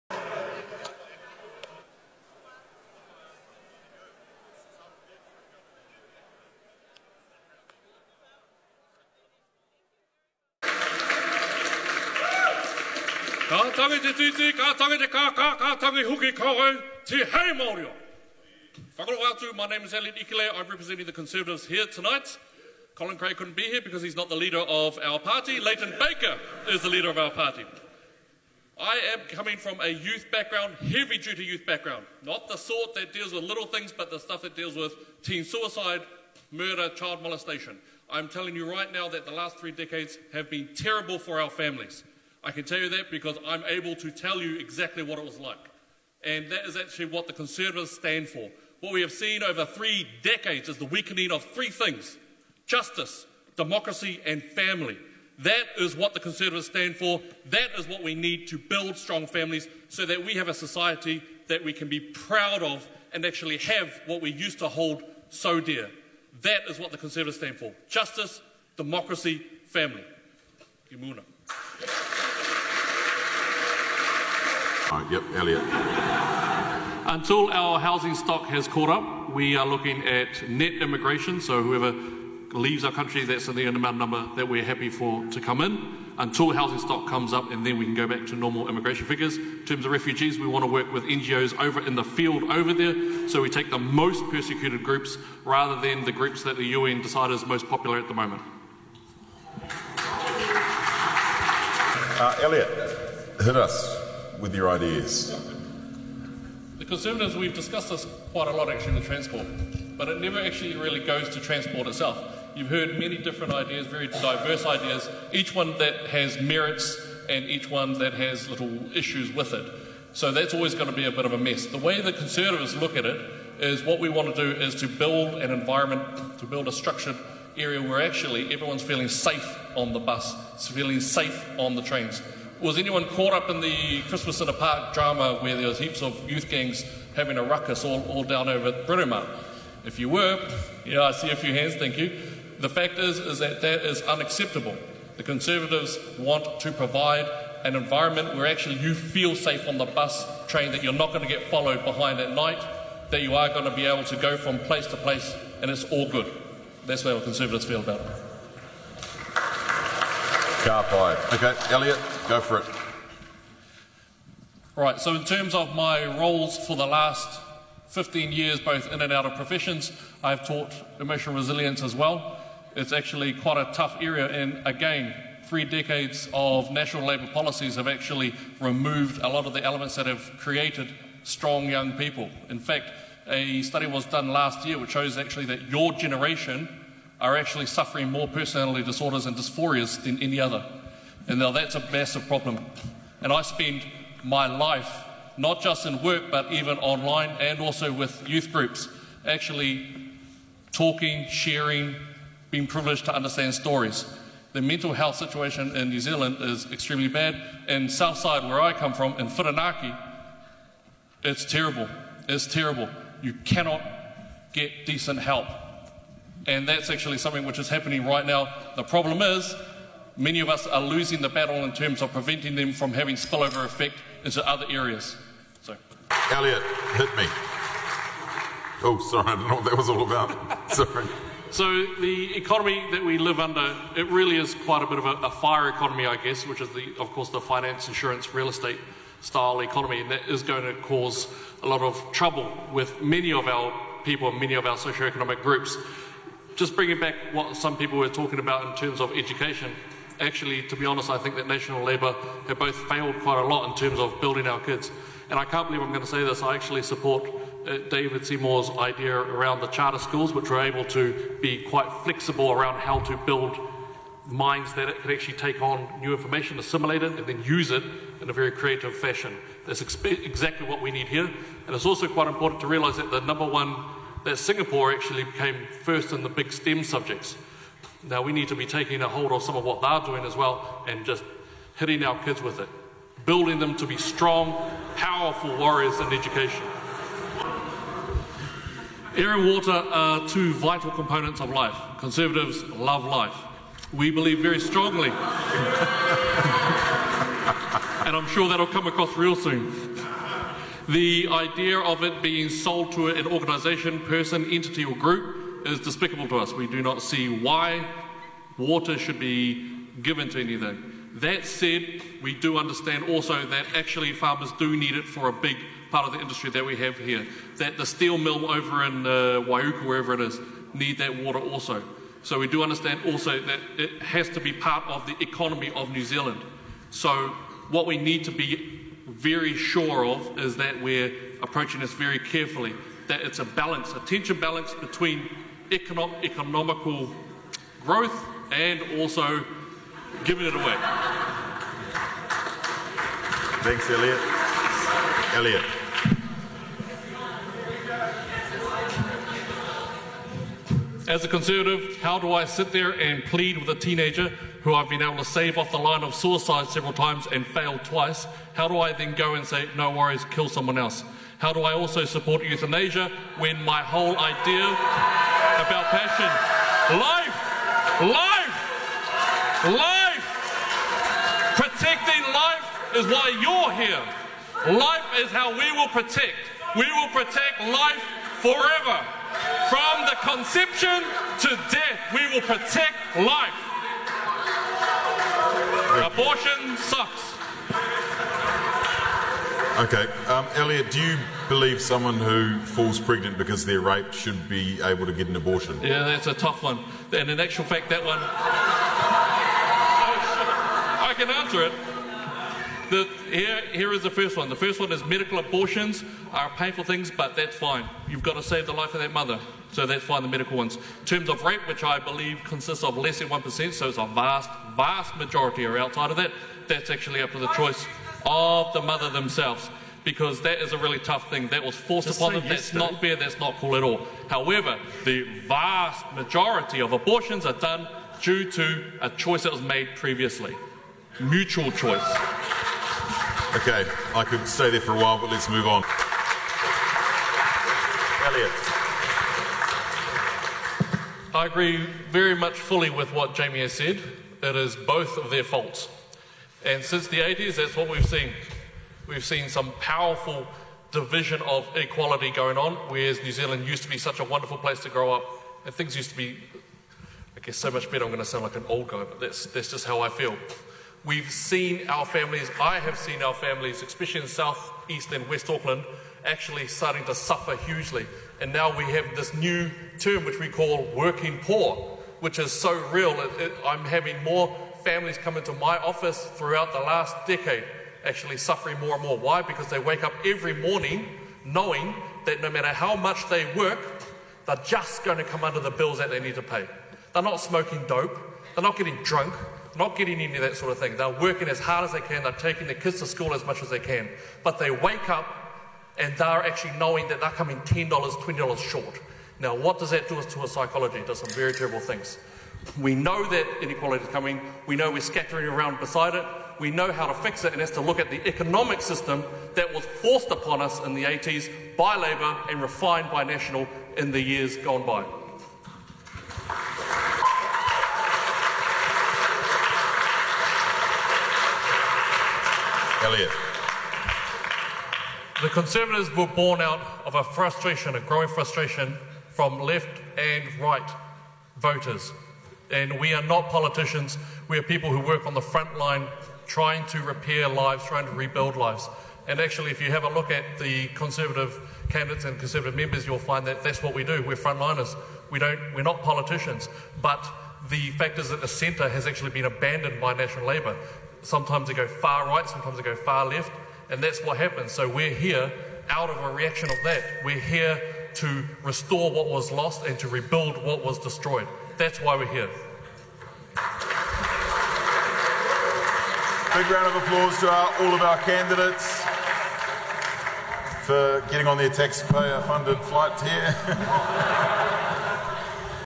Elliot Ikilei at the MCC Debate
Watch Elliot Ikilei represent the Conservative Party at the MCC Auckland University political debate March 3rd 2017.